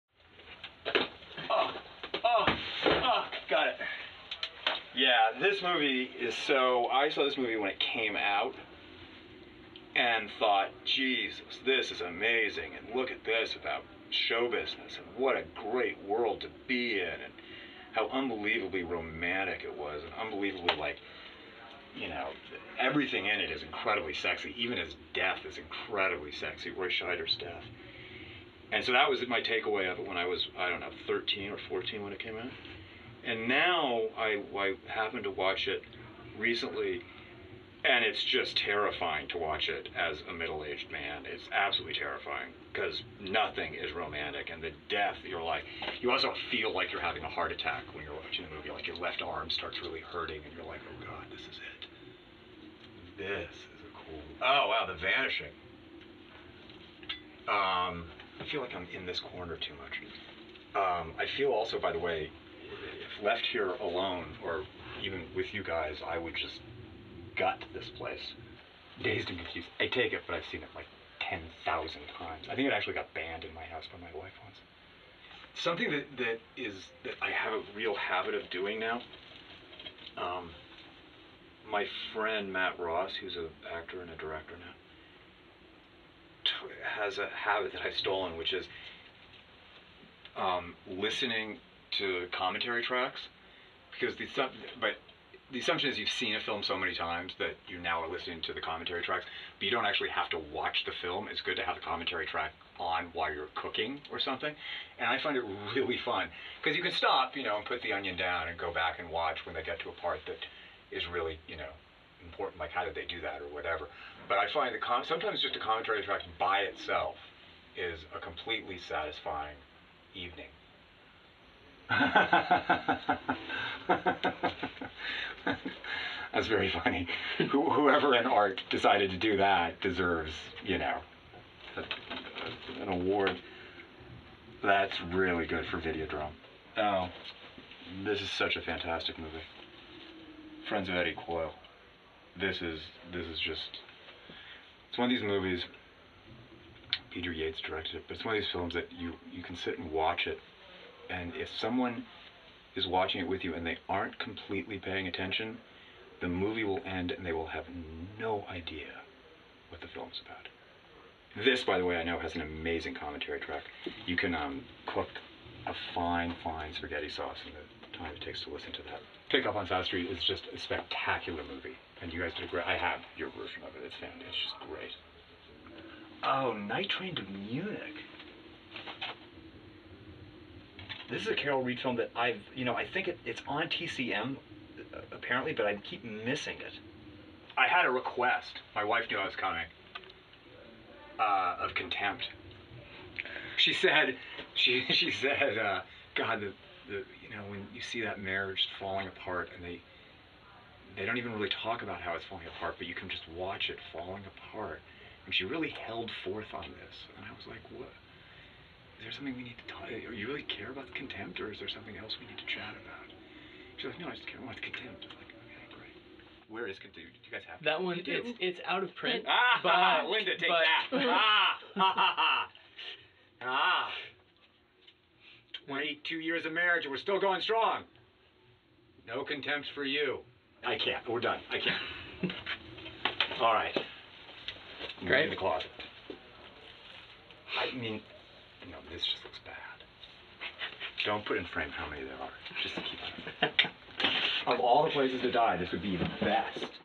Chris Eigeman's DVD Picks
In conjunction with the release this week of A Whit Stillman Trilogy, our box set including METROPOLITAN, THE LAST DAYS OF DISCO, and BARCELONA, actor-director Chris Eigeman stopped by for a trip inside the Criterion closet.